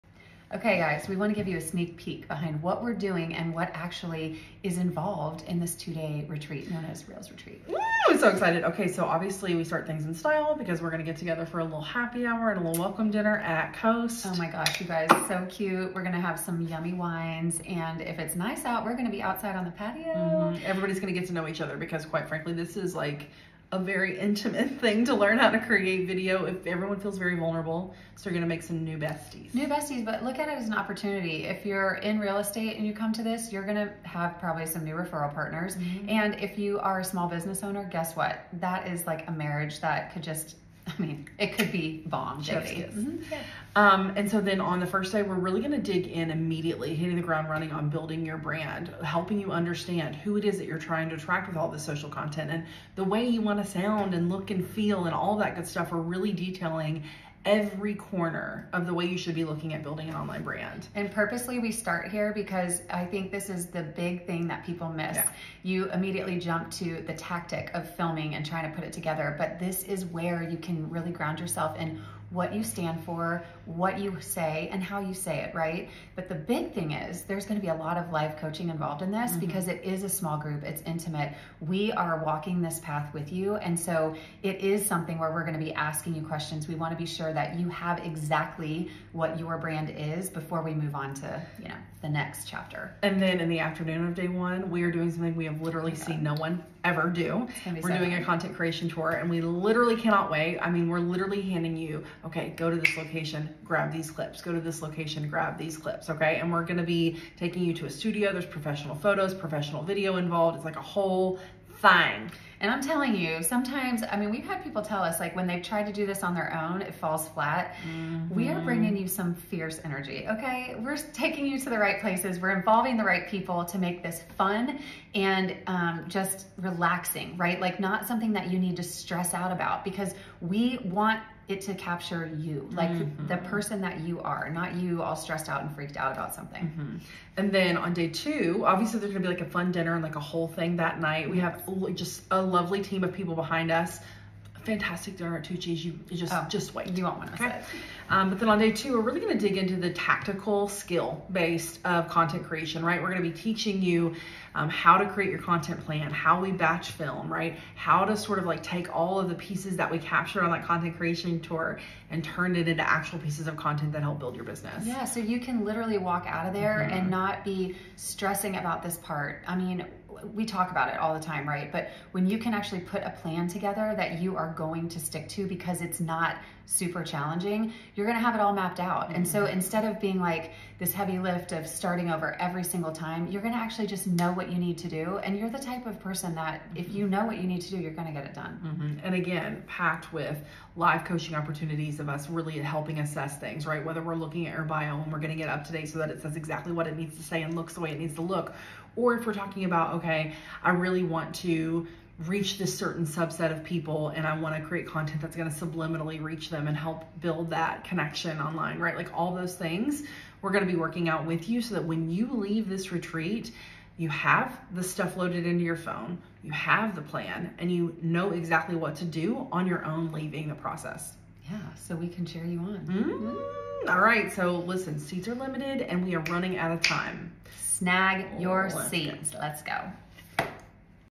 REELS RETREAT 2024
The details on our first ever REELS RETREAT, happening June 18-20.